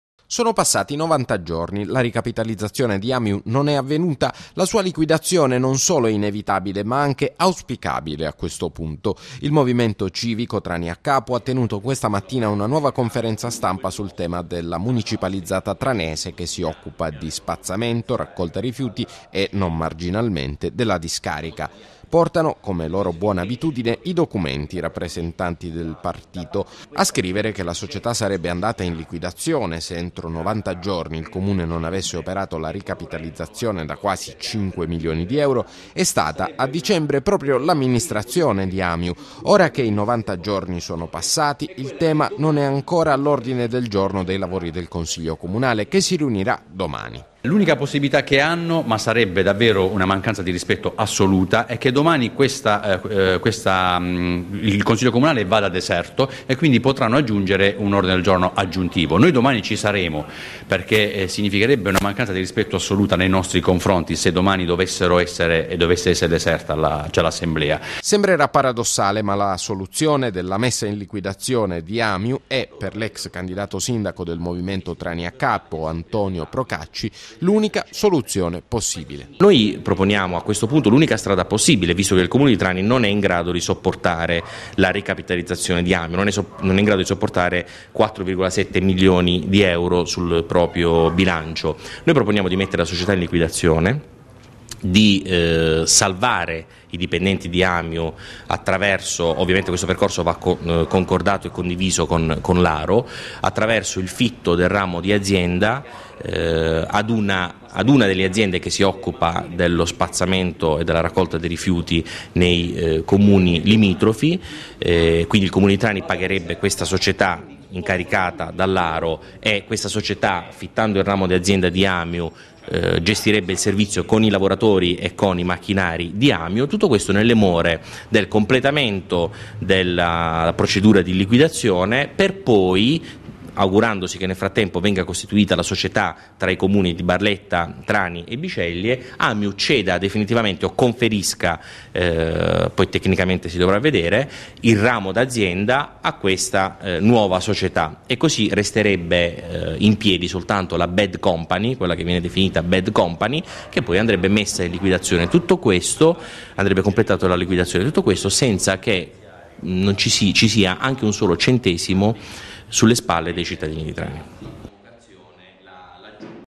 Trani | L'Amiu va messa in liquidazione: la proposta shock del movimento “Traniacapo”
L'Amiu di Trani va messa in liquidazione: la proposta è del movimento civico “Traniacapo”, che tira le somme di tre mesi passati senza che nulla succedesse sulla ricapitalizzazione della municipalizzata che si occupa della raccolta rifiuti.